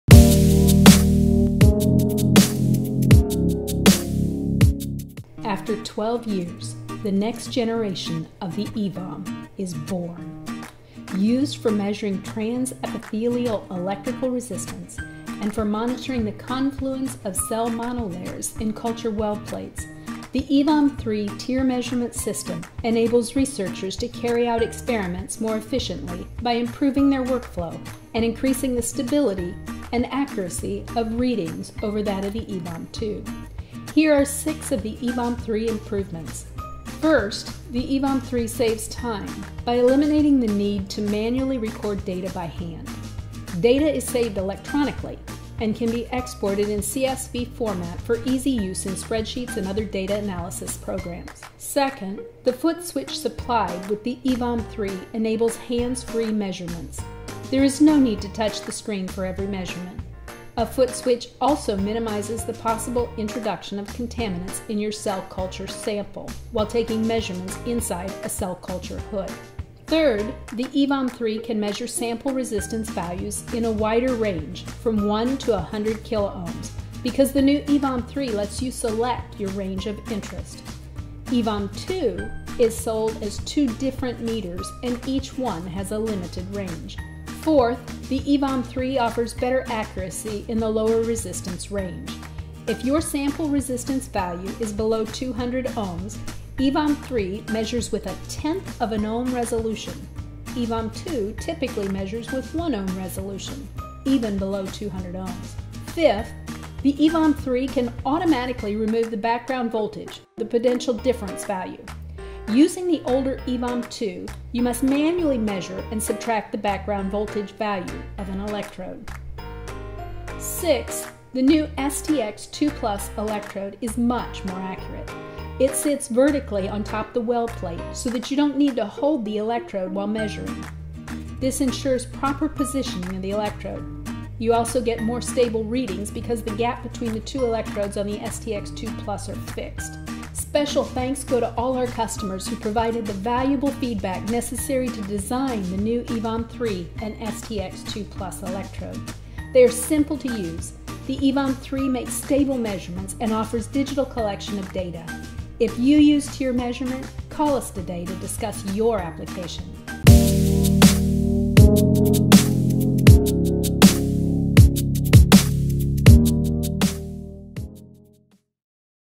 Why Choose an EVOM3 Over an EVOM2 for TEER Measurement
WPI introduces the newest epithelial volt/ohm meter, the EVOM3. Used for measuring Transepithelial Electrical Resistance and monitoring the confluence of cell monolayers in culture well plates. The EVOM3 TEER Measurement System enables researchers to carry out experiments more efficiently by improving the workflow, and increasing the stability and accuracy of readings over that of the EVOM2.  Here are six of the EVOM3 improvements that you will love.